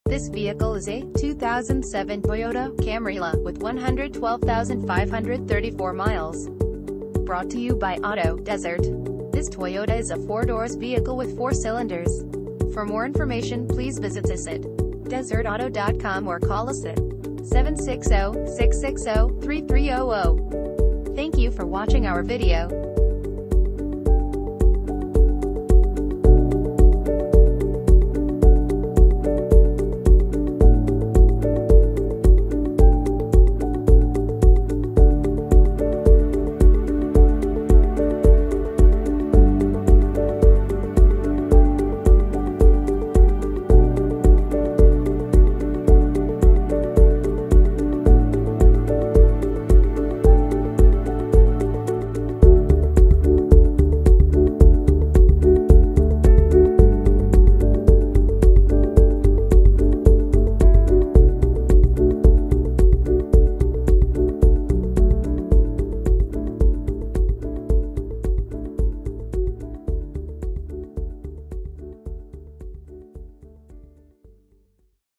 2007 Toyota Camry - Auto Desert
Auto Desert
74990 Joni Dr Unit A1

2007 Toyota  Camry

Clean title, Extended Warranty Available, 112,534 miles, Automatic, Super White exterior, Sedan, FWD: Front Wheel Drive, 4 doors, 4 cylinders, Gasoline, MPG City 24 and 33 in Hwy.